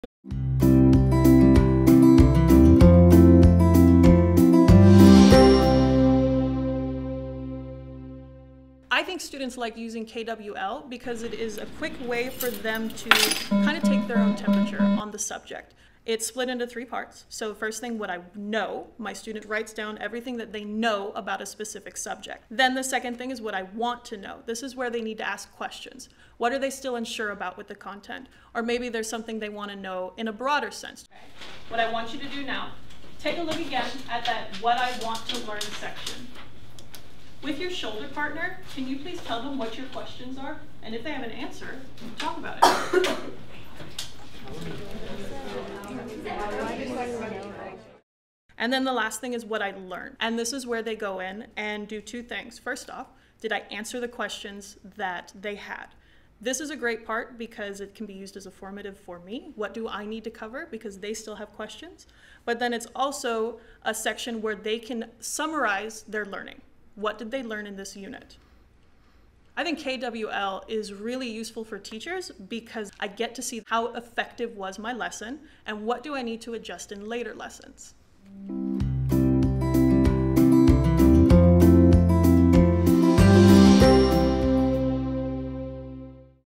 Teacher Toolkit: KWL (High School)
See how Kaitlynn Andre, high school science teacher, uses KWL as a formative assessment tool with her students. The KWL Chart starts students thinking about what they Know about a topic, what they Want to know, and what they have Learned in the end. This note-taking device guides students through a three-step process to activate background knowledge, develop a purpose for learning, and summarize.